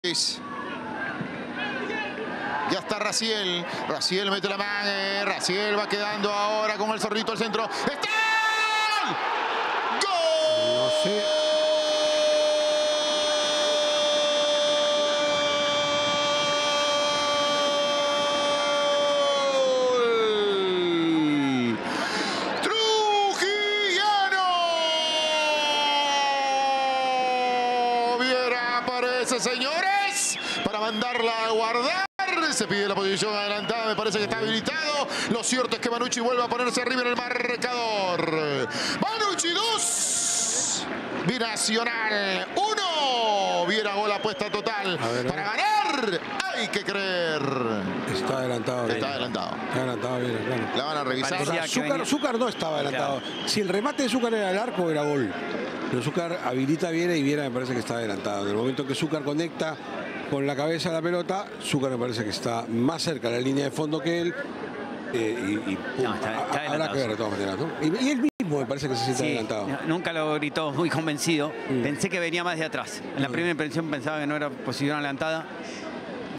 Torneo Clausura: Gustavo Viera marcó el Carlos Mannucci 2-1 Binacional
¡REMONTAN EL PARTIDO! Tras una un centro, Gustavo Viera ⭐ anotó para Carlos Mannucci 🔵⚪🔴 y logró dar vuelta al encuentro ante Deportivo Binacional 🔵. El equipo 'Carlista' no desaprovechó la ocasión y vuelve a complicar al equipo del altiplano en el TorneoClausuraXGOLPERU 🏆🇵🇪🔥.